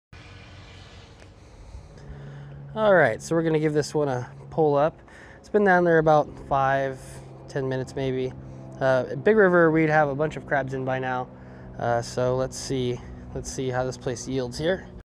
going for rock crabs today definitely got what we came for.
MUSHROOM AND FISHING How to E-BOOKS
- went out to catch some rock crab and definitely got what we came for can't wait to get out and go fishing on the ocean again!!